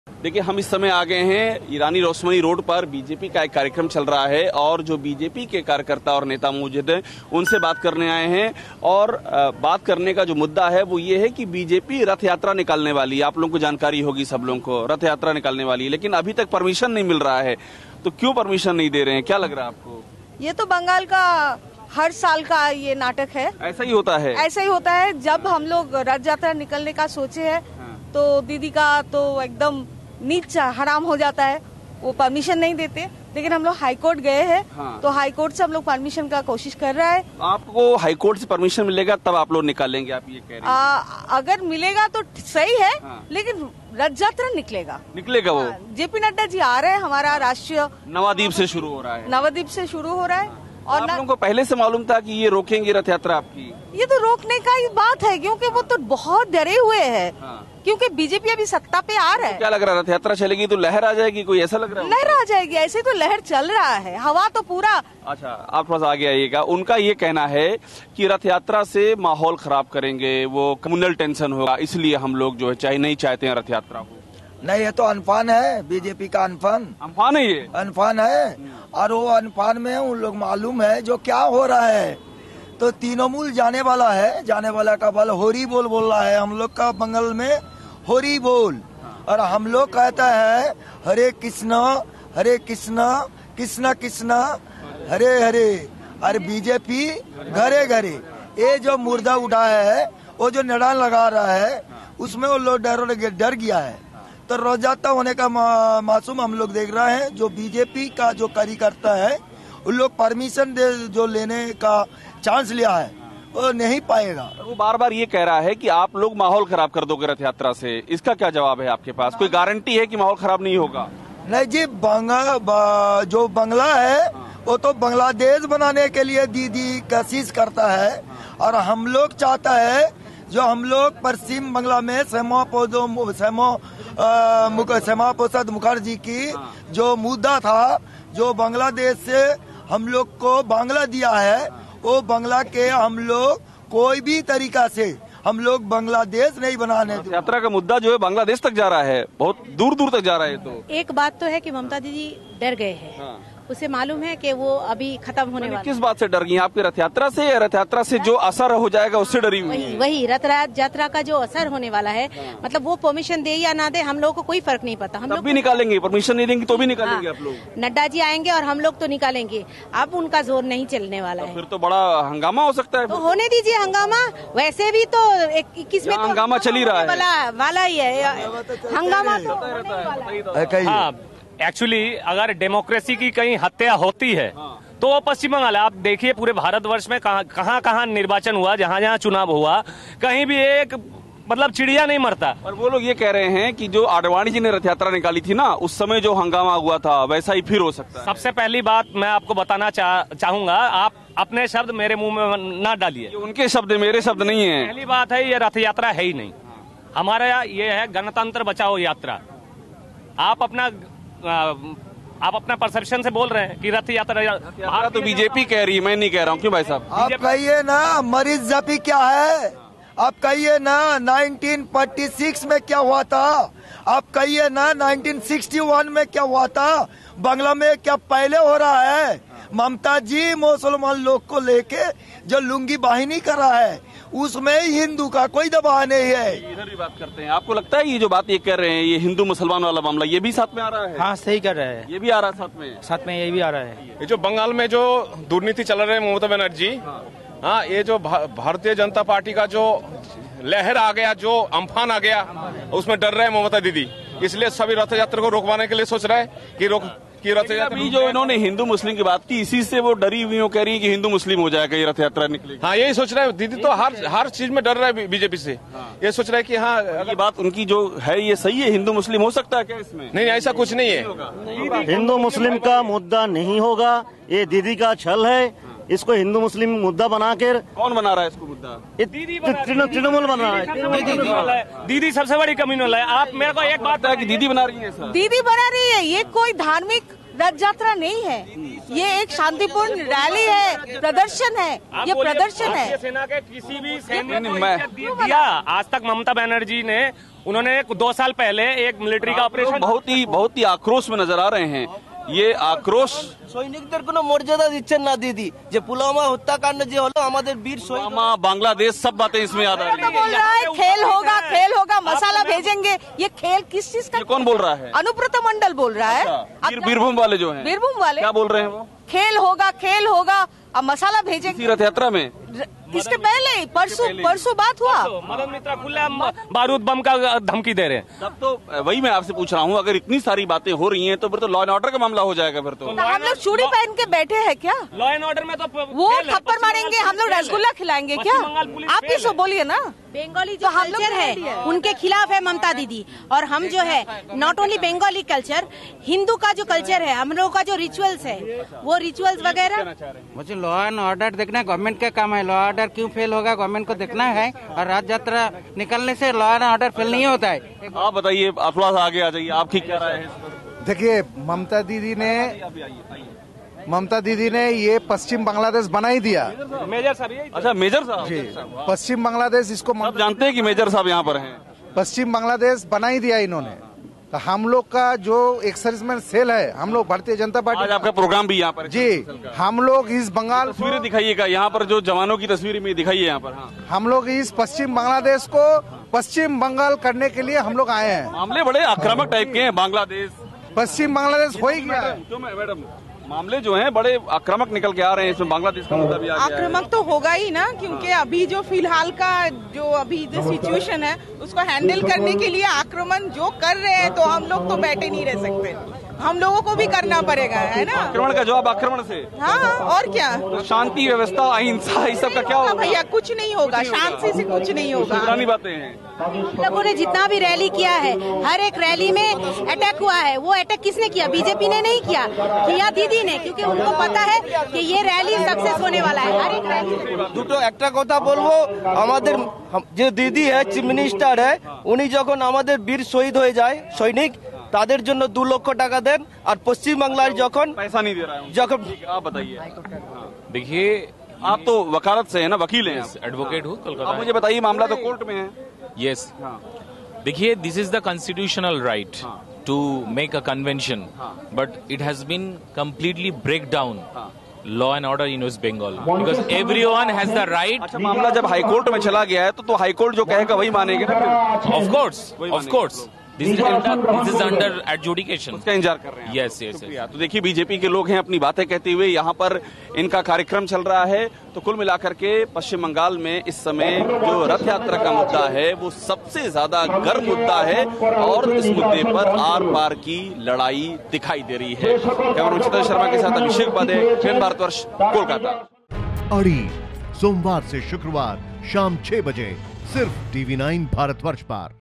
West Bengal : दीदी Vs मोदी का रण..महायुद्ध का अगला चरण ! | बंग युद्ध
पश्चिम बंगाल की राजनीति हर रोज नई करवट ले रही है.TMC छोड़कर जाने वाले नेताओं की लिस्ट काफी लंबी हो गई.ज्यादातर बागी नेताओं ने बीजेपी का दामन थाम लिया है.वहीं दूसरी ओर ममता बनर्जी लगातार केंद्र सरकार पर हमलावर हो रही हैं.BJP ने ममता बनर्जी के किले में सेंध लगाने के लिए राज्य की 294 विधानसभा सीटों पर रथयात्रा निकालने की योजना बनाई है. वहीं मुख्यमंत्री ममता बनर्जी किसी भी सूरत में इस रथयात्रा को रोकना चाहती हैं.
Credit : National​​ | AbhishekUpadhyay​​